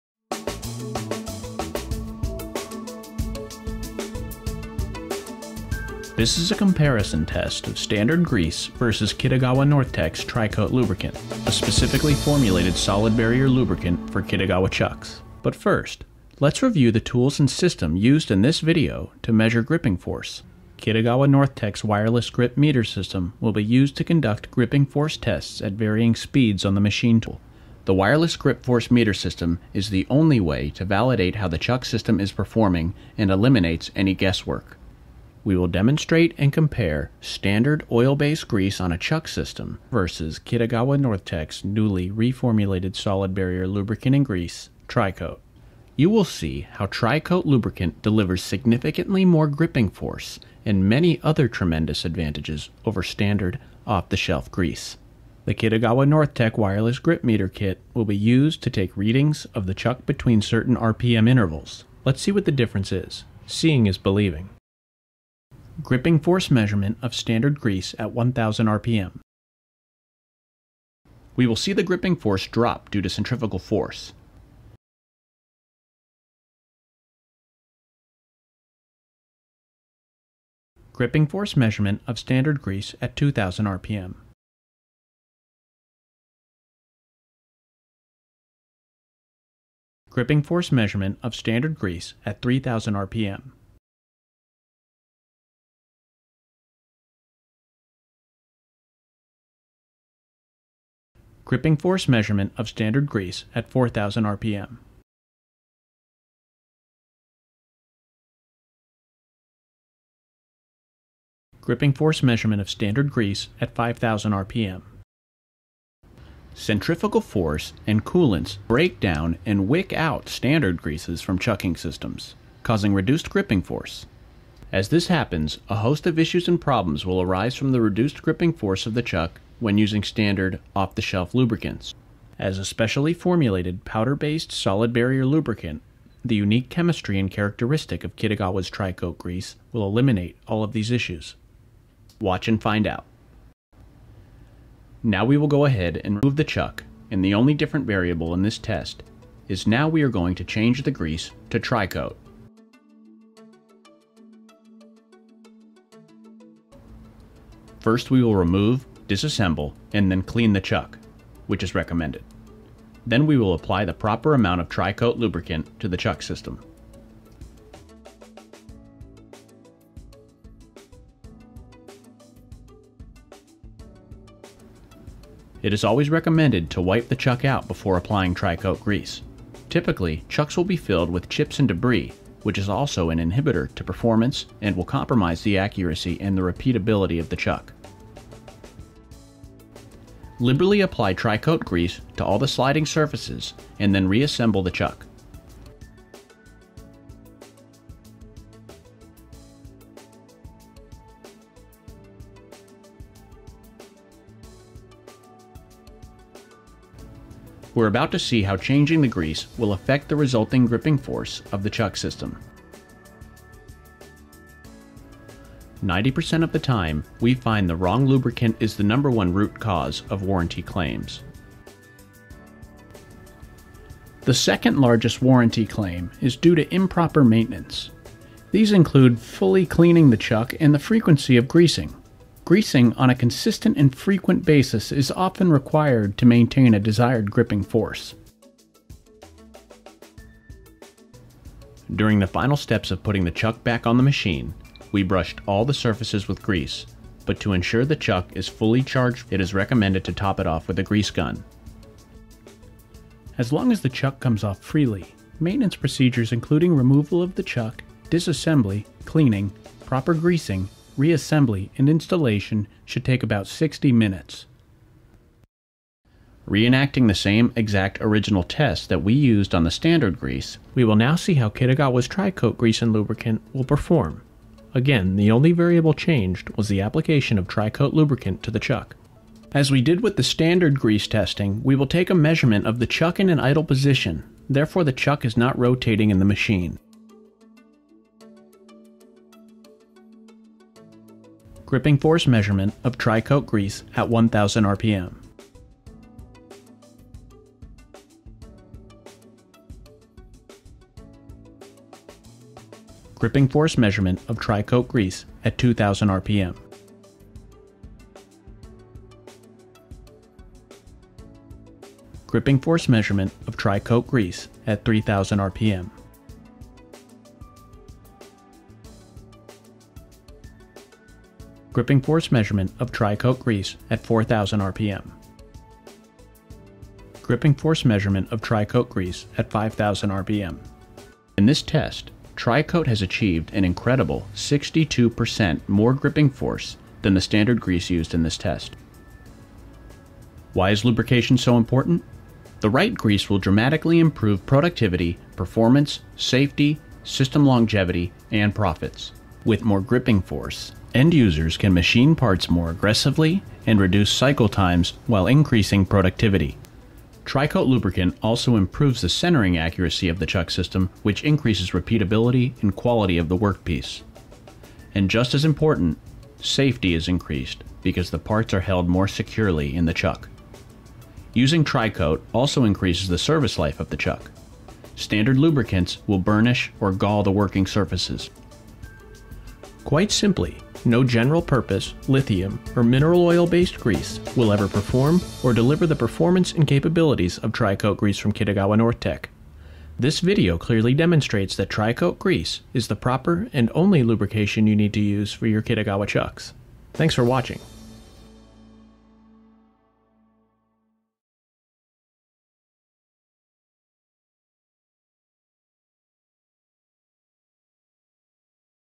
Tri-Kote™ Lubricant for Power Chucks Video Demonstration
Utilizing Kitagawa NorthTech's Wireless Grip Meter Kit, this video demonstrates Tri-Kote™ Lubricant for power chucks versus standard grease in a gripping force test comparison on a chuck system in a CNC machine tool. The results show Kitagawa's new Tri-Kote™ Grease delivers superior performance with over 60% more gripping force than the off-the-shelf lubricant. It also reviews proper maintenance, cleaning, assembly, dis-assembly and lubrication of non-sealed chucks for machine shops.